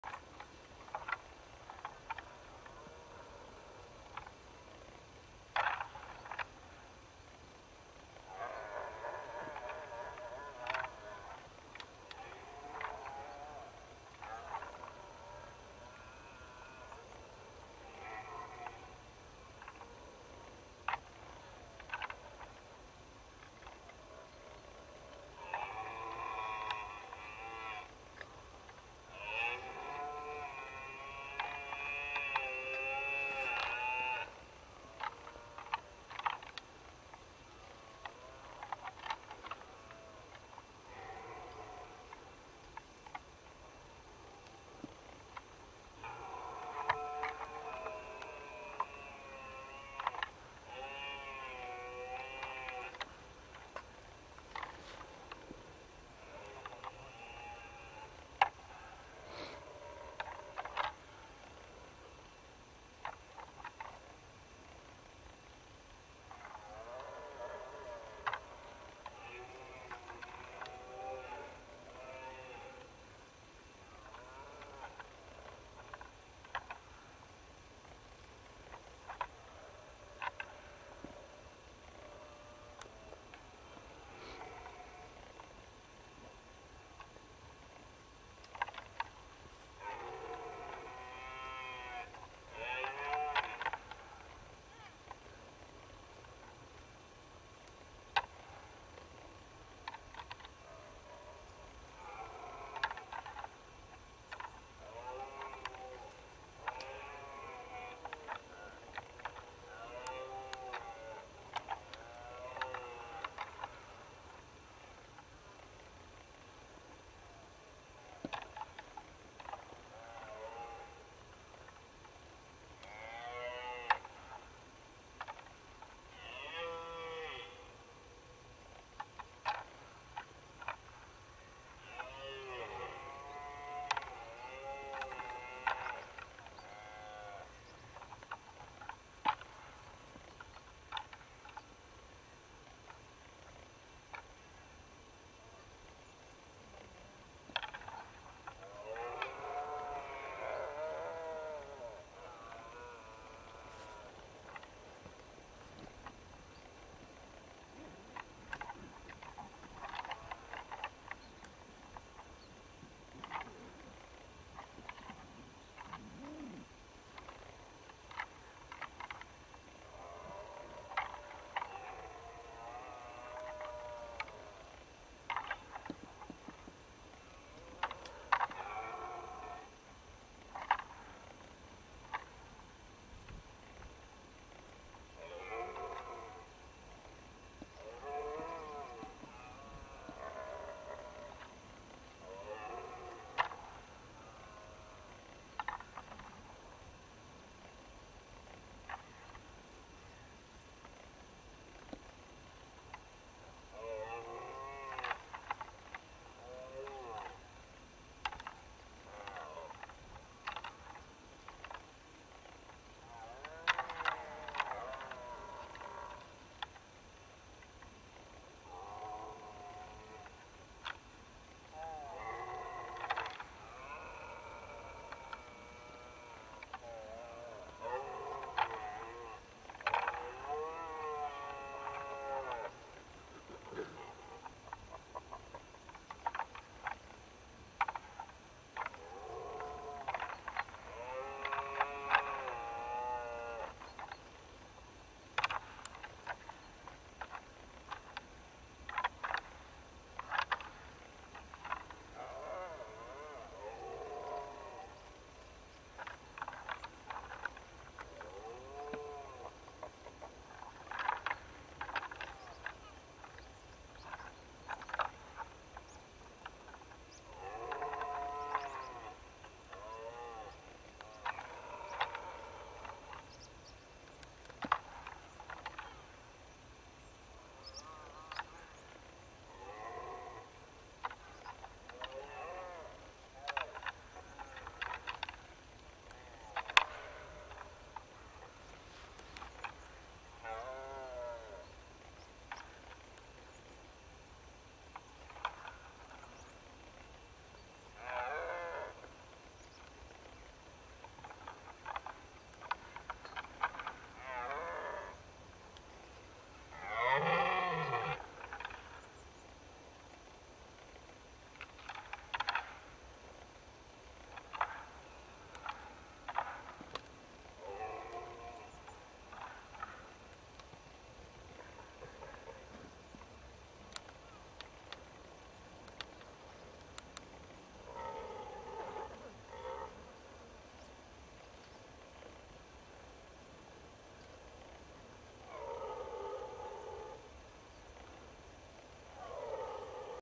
RED DEER RUT Stag Fight Of A ( LIFETIME!!!!!!! )Post 2021!! Filmed 2003
Well this is the best fight I’ve ever caught on film 🎥! Over 24years ! The two stags fighting at first were the two main master stags ! Then stag that came into the fight half way through was the main old master stag for several years ! But as you see didn’t stand a chance cause of his age !! But the stag that won this fight was his first domint year as master stag of this location!!! So for the fascinating facts behind fight makes it even more special to of captured this ! Hope you enjoy it ! Thanks again from 🦌OH 🦌👍